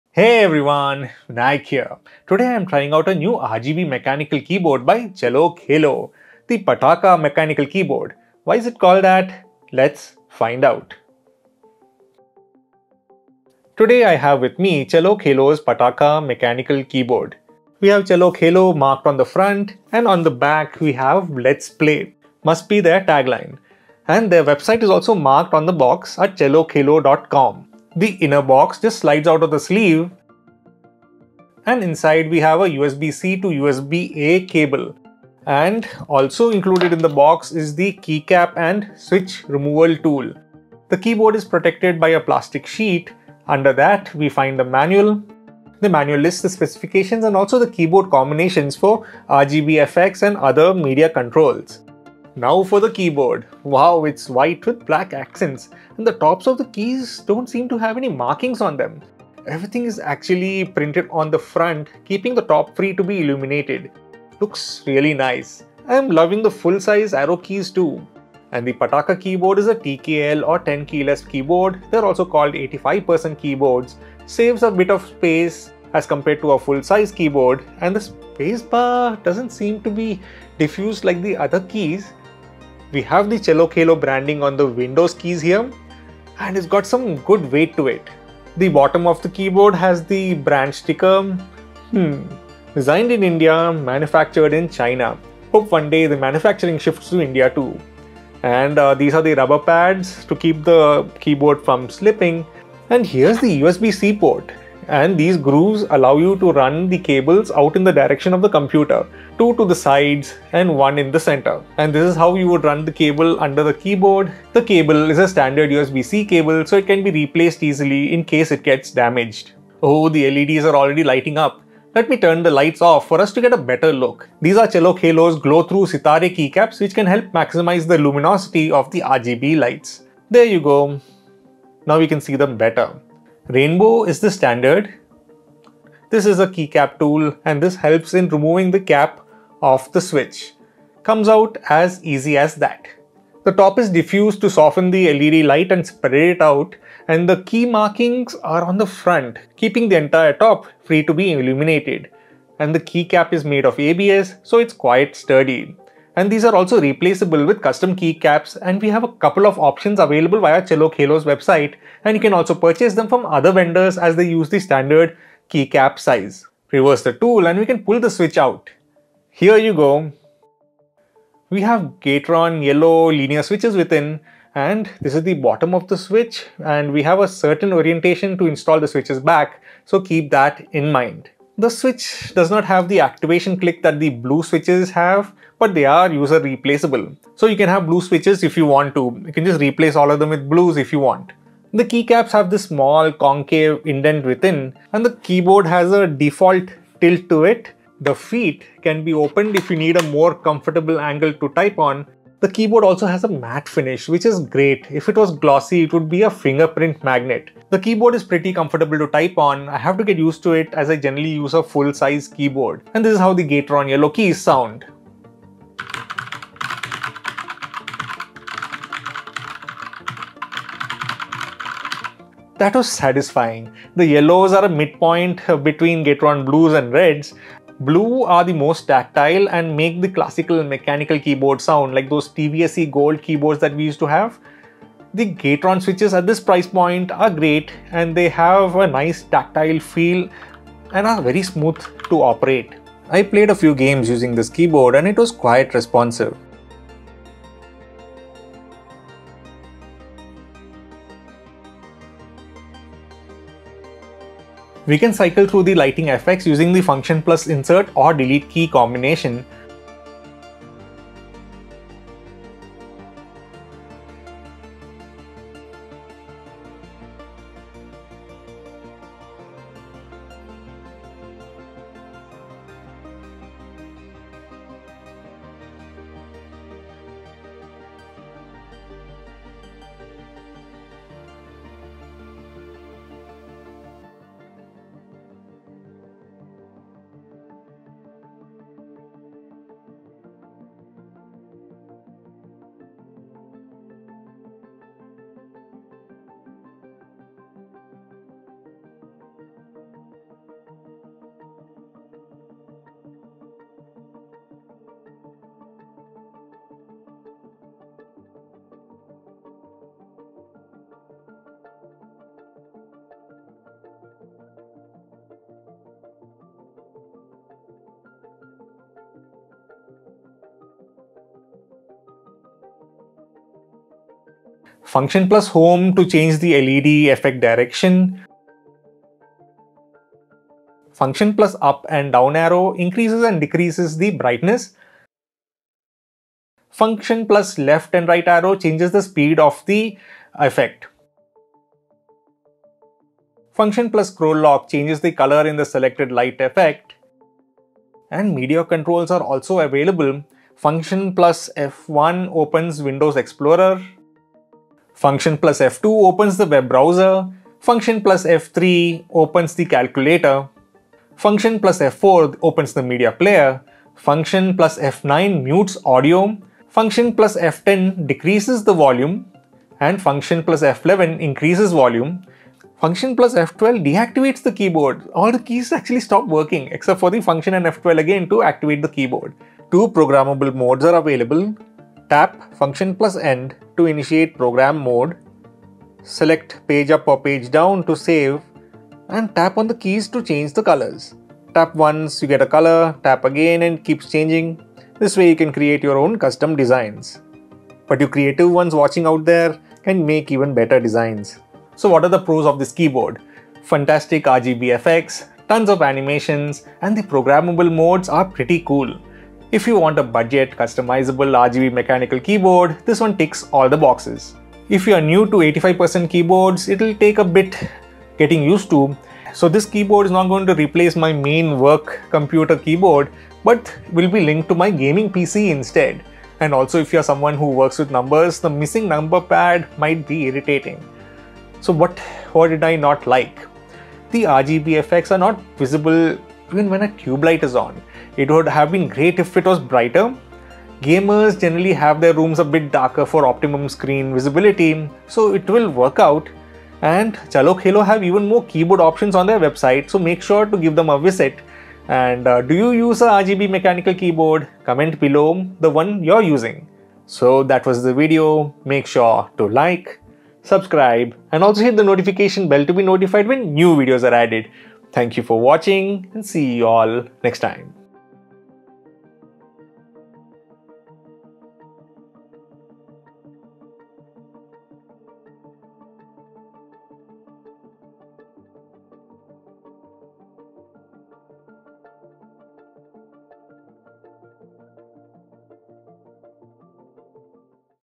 Chalo Khelo Pathaka Gaming RGB Keyboard
Today I'm trying out a new RGB mechanical keyboard by Chalo Khelo, the Pataka mechanical keyboard. Why is it called that? Let’s find out.

⏰ Timecodes ⏰